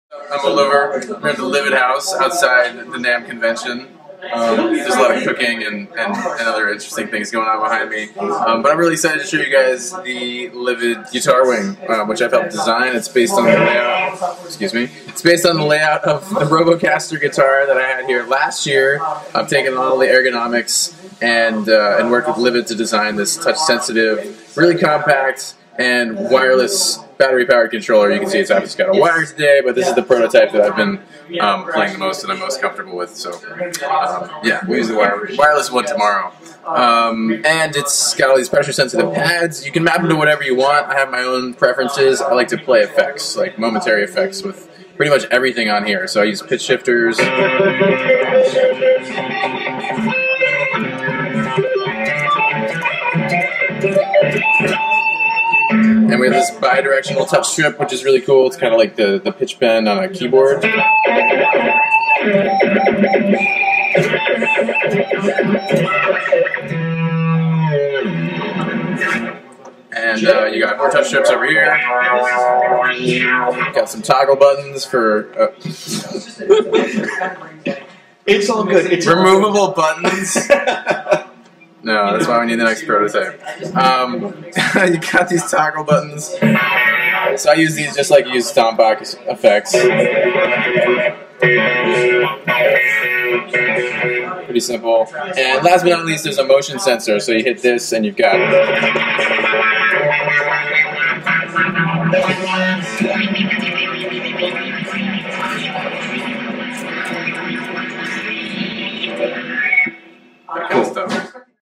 NAMM 2014: Livid Guitar Wing with Moldover
Controllerism king and perennial MIDI tweaker Moldover takes some time out to show us the Livid Guitar Wing prototype (being the key word) at the Livid House at NAMM 2014.